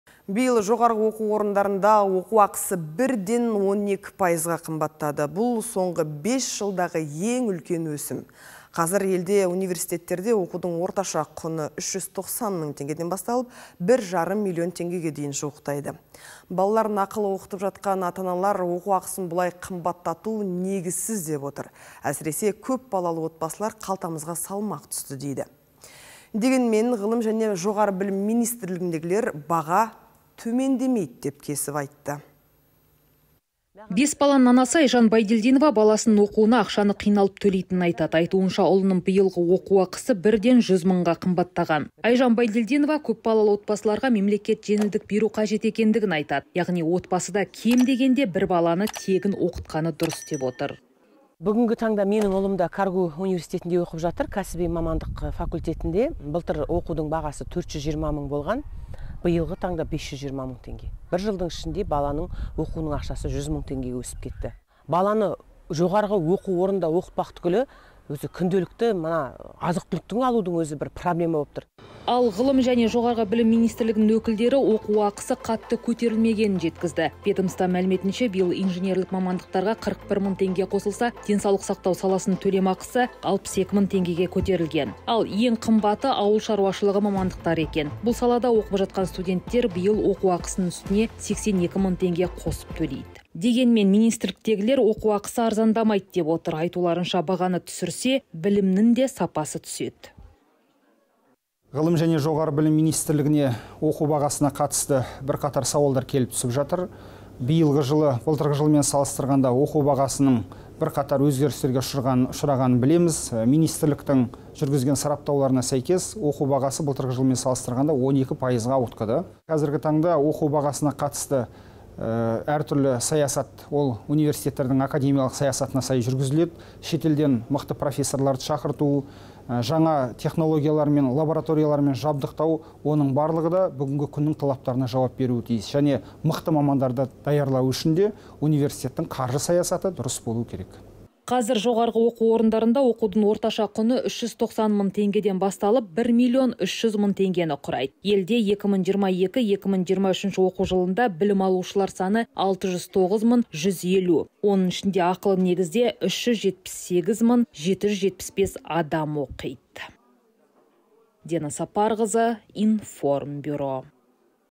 Баға төмендемейді! Университеттерде оқу ақысы биыл 12 пайызға қымбаттады
Биыл жоғары оқу орындарында оқу ақысы бірден 12 пайызға қымбаттады. Бұл соңғы бес жылдағы ең үлкен өсім. Қазір елде университеттерде оқудың орташа құны 390 мың теңгеден басталып, 1,5 миллион теңгеге дейін жуықтайды. Балаларын ақылы оқытып жатқан ата-аналар оқу ақысын бұлай қымбаттату негізсіз деп отыр. Әсіресе көпбалалы отбасылар қалтамызға салмақ түсті дейді. Дегенмен Ғылым және жоғары білім министрлігіндегілер баға төмендемейді деп кесіп айтты.
Бес баланың анасы Айжан Байгильдинова баласының оқуына ақшаны қиналып төлейтінін айтады. Биылғы жылы оқудың ақасы бірден жүз мыңға қымбаттағын айтады. Оның айуынша, көпбалалы отбасыларға мемлекет жеңілдік беру қажет. Яғни отбасы да кем дегенде бір баланы тегін оқытқаны дұрыс болатынын айтады.
АЙЖАН БАЙДЕЛЬДИНОВА, Қарағанды қаласының тұрғыны:
– Бүгінгі таңда менің ұлым да КарГу университетінде оқып жатыр. Кәсіби мамандық факультетінде. Былтыр оқудың бағасы 420 мың болған, биылғы таңда 520 мың теңге. Бір жылдың ішінде баланың оқуының ақшасы 100 мың теңгеге өсіп кетті. Баланы жоғары орында оқытпақ түгілі, күнделікті азық-түліктің алу өзі проблема боп тұр.
Ал Ғылым және жоғары білім министрлігінің өкілдері оқу ақысы қатты көтерілмегенін айтады. Ведомства мәліметше, биыл инженерлік мамандықтарға 41 мың теңге қосылса, денсаулық сақтау саласының төлемақысы 62 мың теңгеге көтерілген. Ал ең қымбаты ауыл шаруашылығы мамандықтары екен. Бұл салада оқып жатқан студенттер биыл оқу ақысының үстіне 82 мың теңге қосып төлейді. Дегенмен, министрліктегілер оқу ақысы арзандамайды деп отыр. Айтуларынша, бағаны түсірсе білімнің де сапасы түседі.
ЕРЖАН ӘМІРБЕКҰЛЫ, ҚР Ғылым және жоғары білім министрлігі департаменті директорының орынбасары:
– Қазіргі таңда оқу бағасына қатысты әртүрлі саясат, ол университеттердің академиялық саясатына сай жүргізіледі. Шетелден мықты профессорларды шақырту. Технология мен лабороториялармен жабдықтау. Оның барлығы да бүгінгі күннің талаптарына жауап беруі тиіс. Мықты мамандарды даярлау үшін де университетің қаржы саясаты дұрыс болу керек.
Қазір жоғары оқу орындарында оқудың орташа құны 390 мың теңгеден басталып, 1 миллион 300 мың теңгені құрайды. Елде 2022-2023 оқу жылында білім алушылар саны 609 мың 150. Оның ішінде ақылы негізде 378 775 адам оқиды.
Диана Сапарқызы